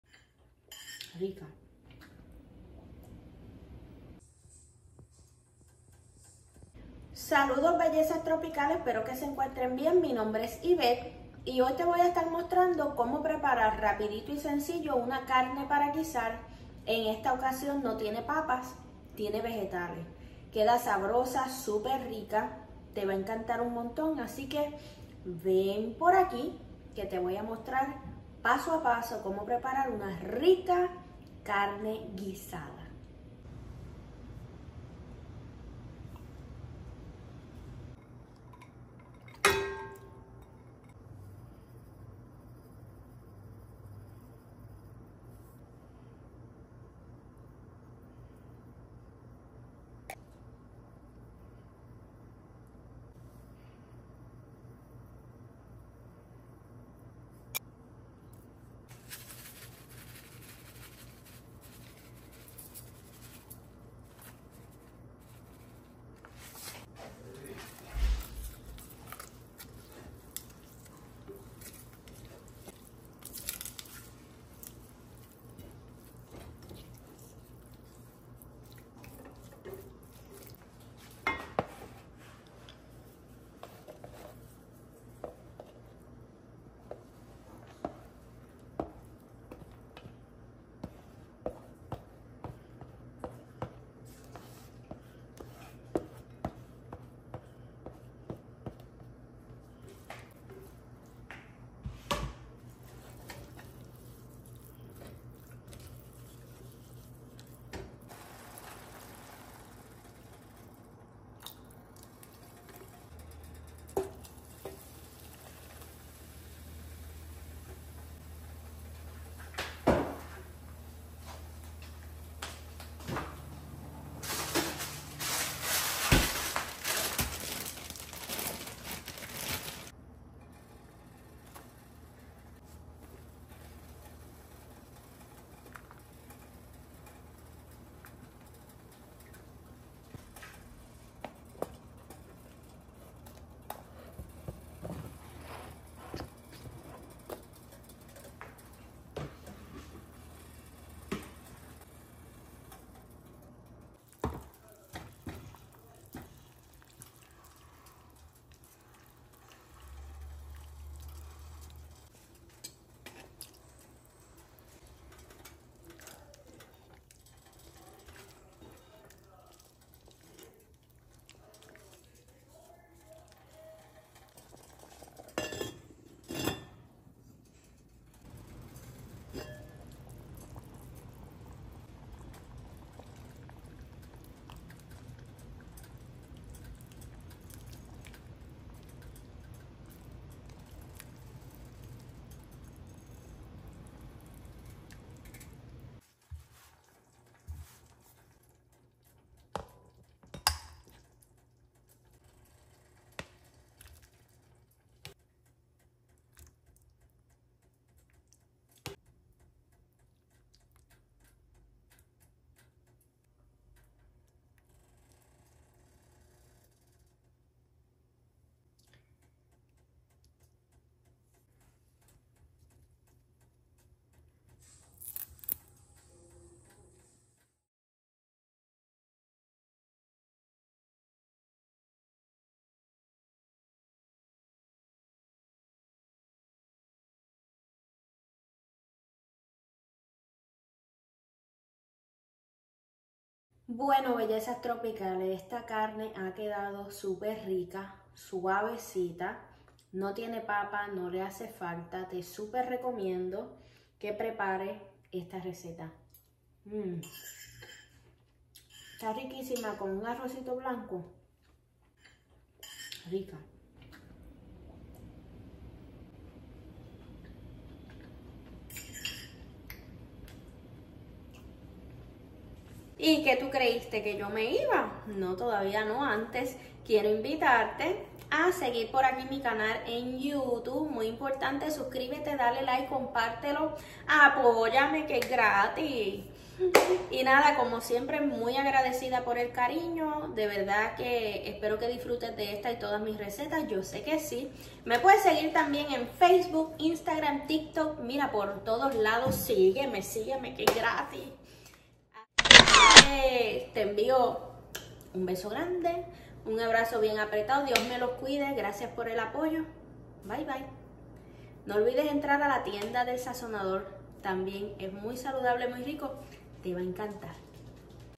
How to make puerto rican beef stew | Carne de res puertorriqueño
Cada vez que preparo esta carne no rinde para nada de tanto que comemos